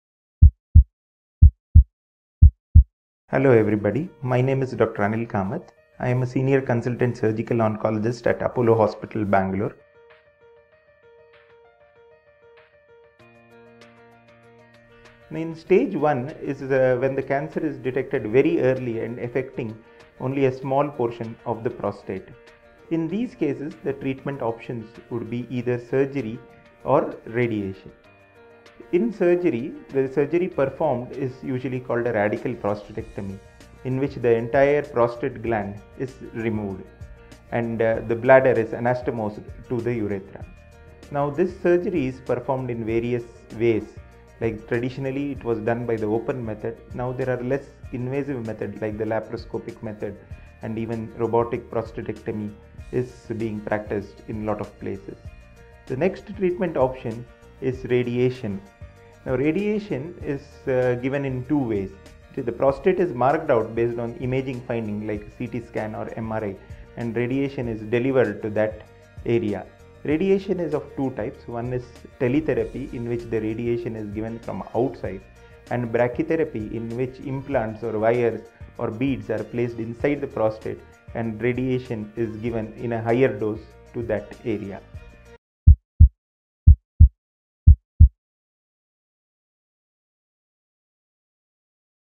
How is stage-1 prostate cancer treated? - Dr. Anil Kamath
In stage one if the cancer is detected very easily because  a small portion is affected due to prostate cancer. In these case the treatment option is either surgery or  radiation. In surgery the surgery performed usually a radical protectorate in which the entire prostate gland is removed and the bladder is anatomists  to the urine.
Now this surgery is performed by various ways like traditionally it was done by the open method and arthroscopic method and by robotics. The next treatment option is radiation. Radiation is given in two ways  if the prostate is marked out based on PT scan or MRI scan.
Radiation is of two types one is teletherapy in which the  radiation is given from out side and second is brachytherapy in which implants or wires placed inside the prostate and radiation is given to that area.